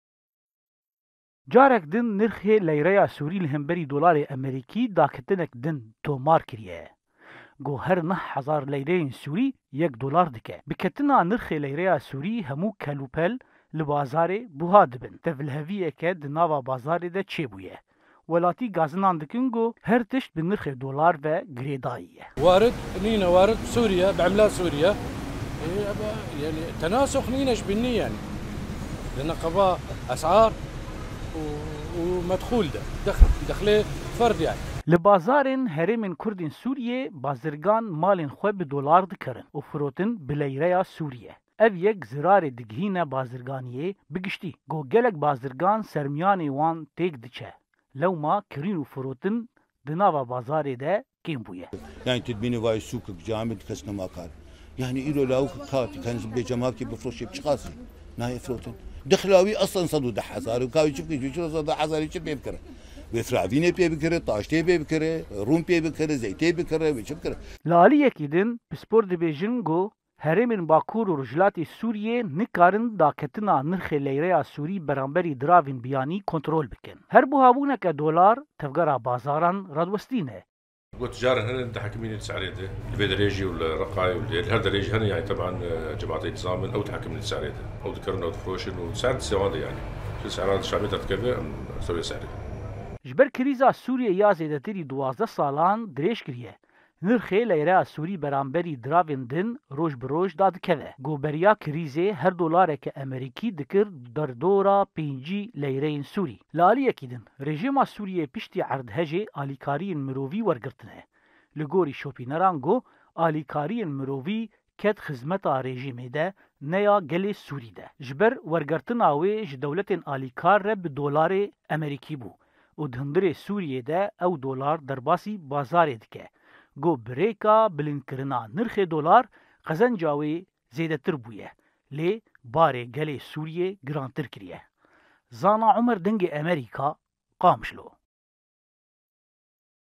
Nirxê Lêreya Sûrî Dadikeve Asta Herî Nizim Ji Dema Krîza Sûrîyê Ve | Dengê Amerîka
Li bazarên herêmên kurdên Sûrîyê bazirgan malên xwe bi dolar dikirin, û firotin bi lêreya Sûrî ye, ev yek zirarê dighîne bazirganîyê bi giştî, ku gelek bazirgan sermyanên wan têk diçin.